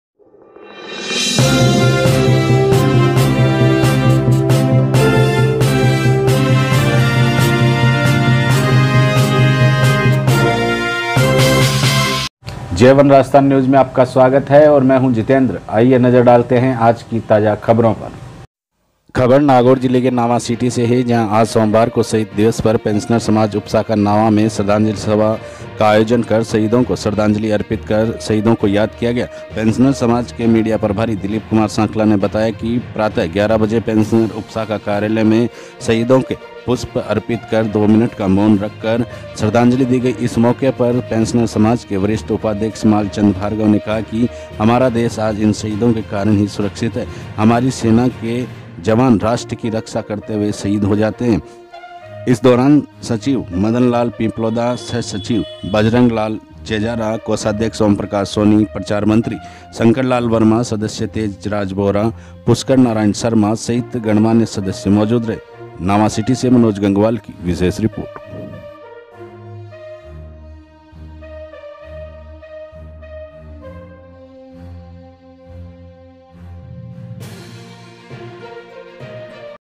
नावासिटी। शहीद दिवस पर पेंशनर समाज ने दी शहीदों को श्रद्धांजलि।
@ मनोज गंगवाल की रिपोर्ट